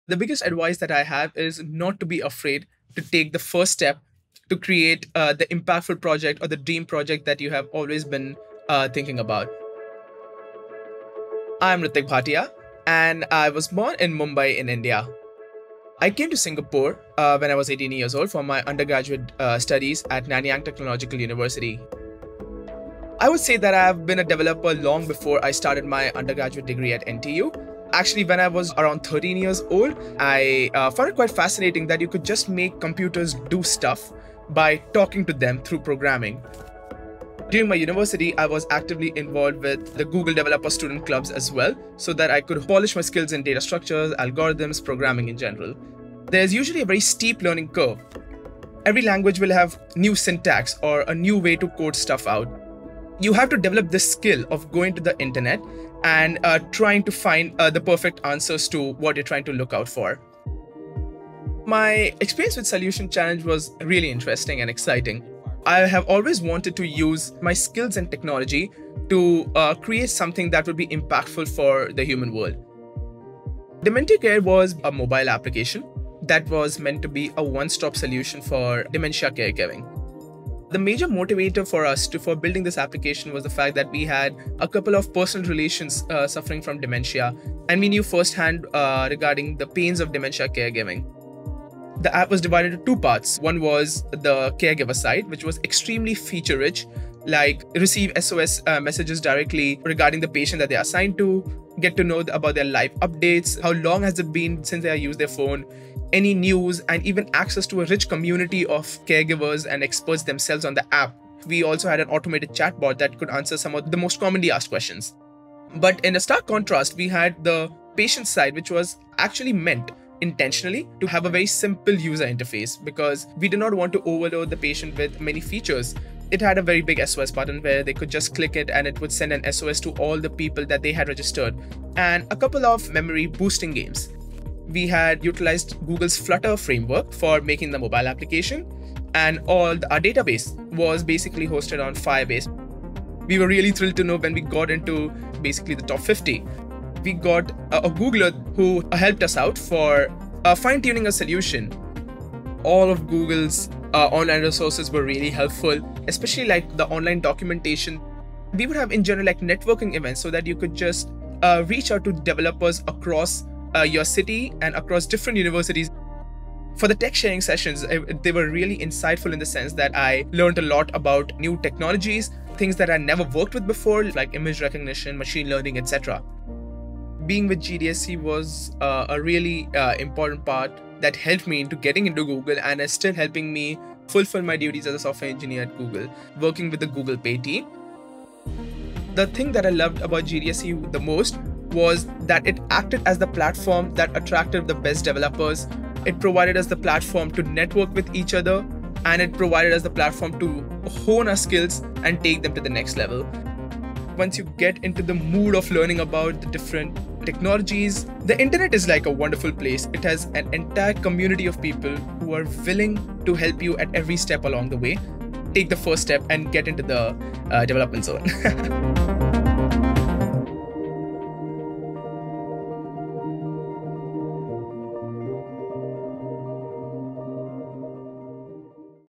Creating a winning app: Ritik’s story as a GDSC member in Singapore
Meet Ritik Bhatia, Software Engineer for Google Pay, former GDSC member and Solution Challenge 2021 winner. Ritik shares how he gained technical skills and found a community of developers through the Google Developer Student Club. Find out more about his winning project, DementiCare, an app that provides a one-stop solution for dementia care. He discusses DementiCare's features and the Google technologies used to build the platform. He also reveals how GDSC served as a stepping stone to finding a career at Google.

Google